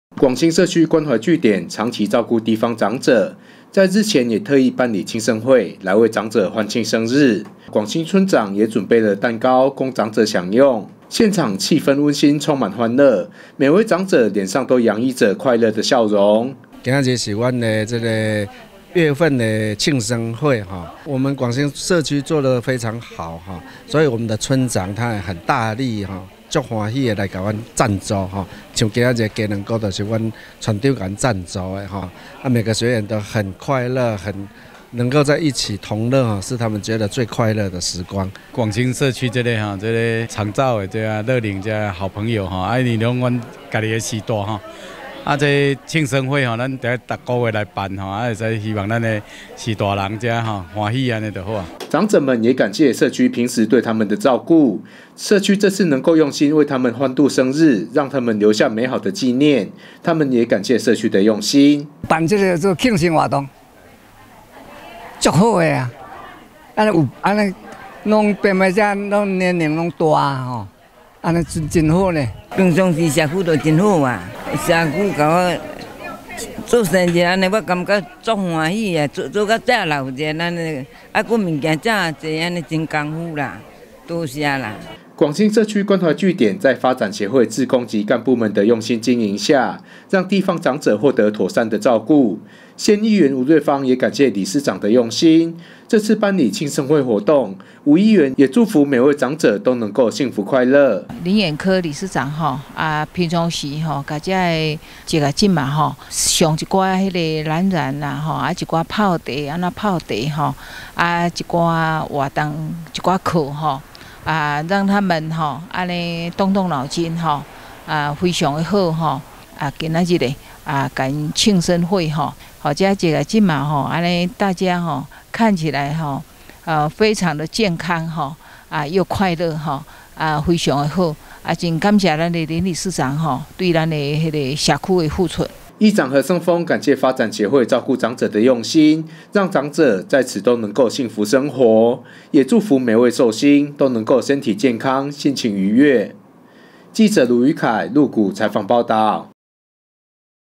20190501 民議新聞 廣興社區辦理慶生會為長者歡慶生日(縣議員 吳瑞芳)
廣興社區關懷據點，長期照顧地方長者使他們生活過得充實愉快，在日前也特意辦理慶生會，來為長者歡慶生日，當天現場氣氛溫馨充滿歡樂，每位長者臉上都洋溢著快樂的笑容。議長何勝豐感謝發展協會照顧長者的用心，讓長者在此都能幸福生活，也祝福每位壽星，都能夠身體健康心情愉悅。